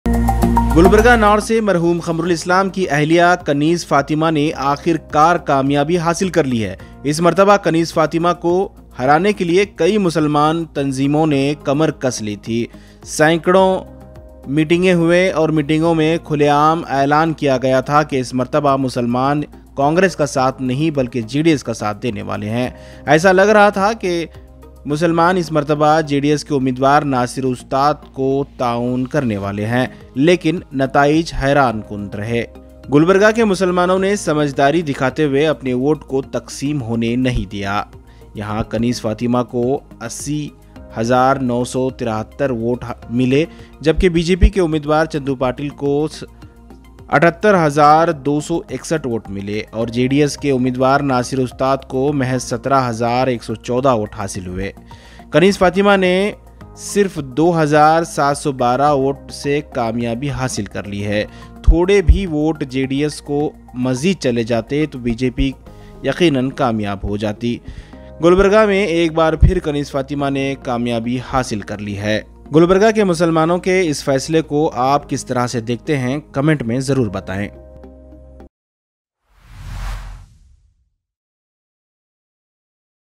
Lone Women Muslim MLA Kaniz Fatima won from Gulberga North
Congress Kalburgi north Candidate Kaniz Fatima won election